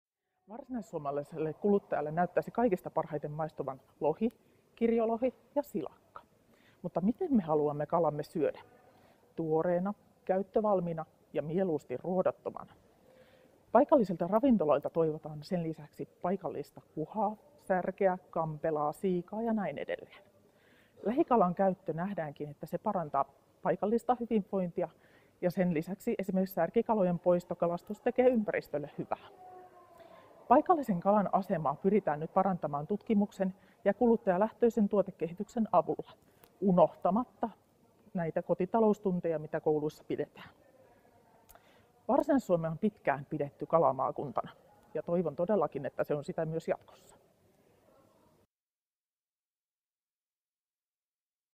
Mitä kalaa Varsinais-Suomessa syödään?
Turun yliopiston Brahea-keskuksessa projektipäällikkönä työskentelevä Mervi Louhivaara kertoo videolla, millaista kalan kulutus on Varsinais-Suomessa; mitä kalaa varsinaissuomalaiset mieluiten syövät ja miten he sen valmistavat.

Brahea-keskus selvitti kesä-heinäkuussa Varsinais-Suomessa kuluttajilta, ammattikeittiöiltä ja kalakauppiailta paikallisen kalan käyttöön liittyviä seikkoja sekä toiveita saariston alueen ravintoloiden kalaruuille. Selvitys toteutettiin osana Paikallisen kalan asiakaslähtöinen tuotekehitys ja arvon lisääminen Turunmaalla -hanketta.